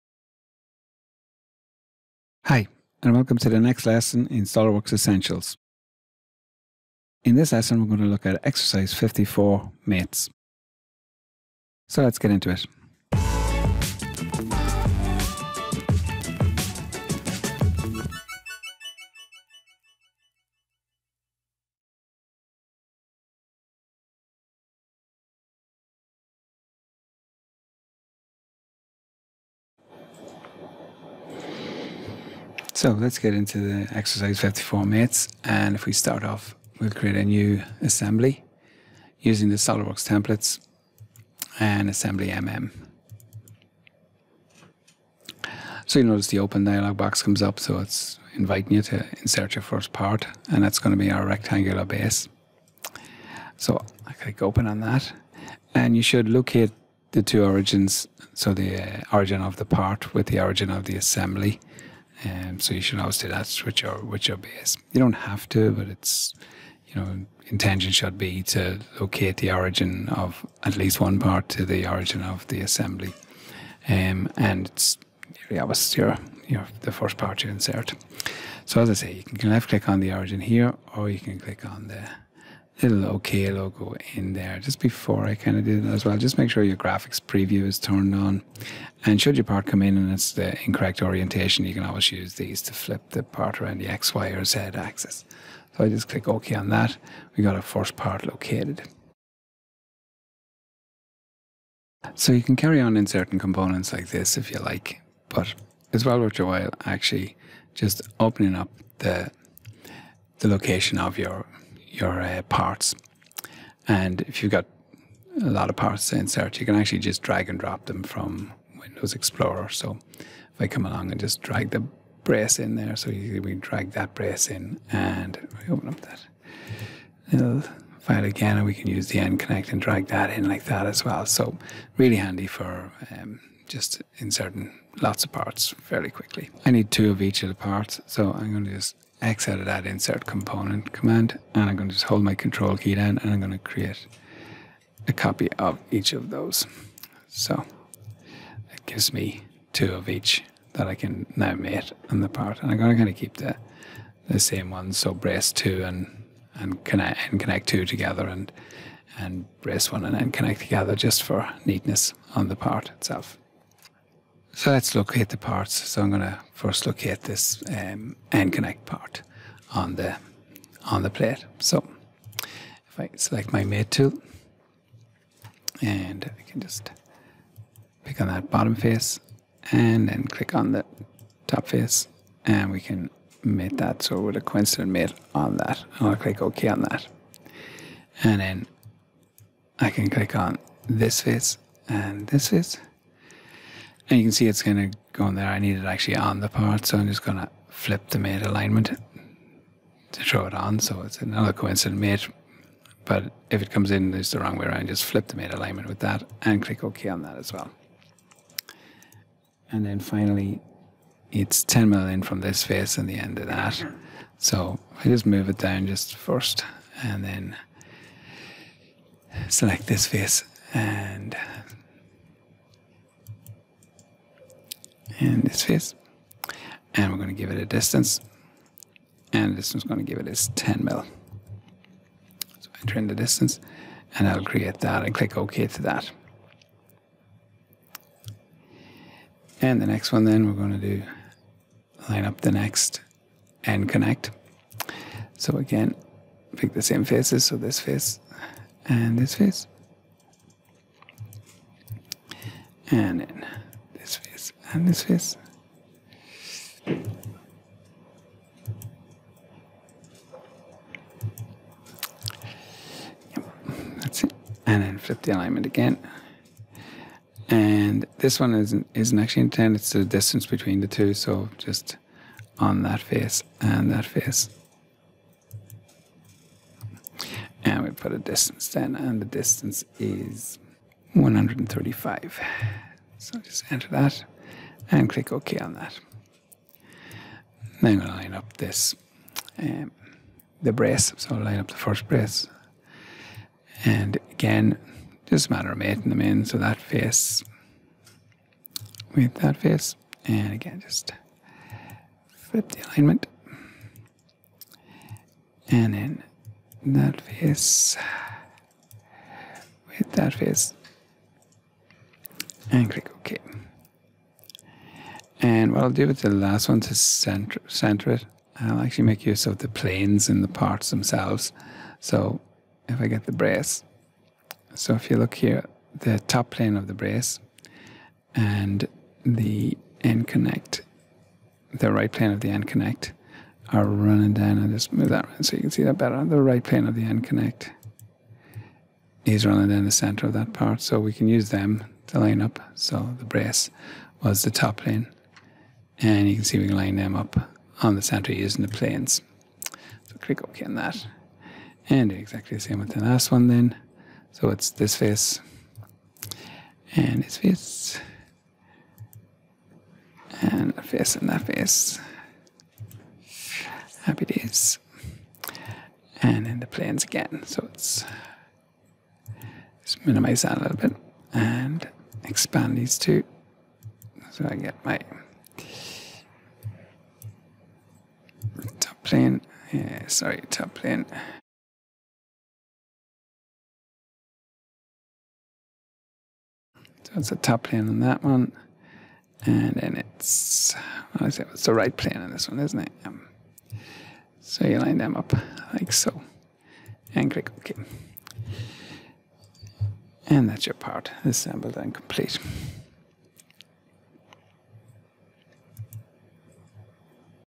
Assembly Modelling Mates
In this exercise you'll learn learn how to create an assembly by adding components to a new assembly and use mates to fix the various parts into their correct positions.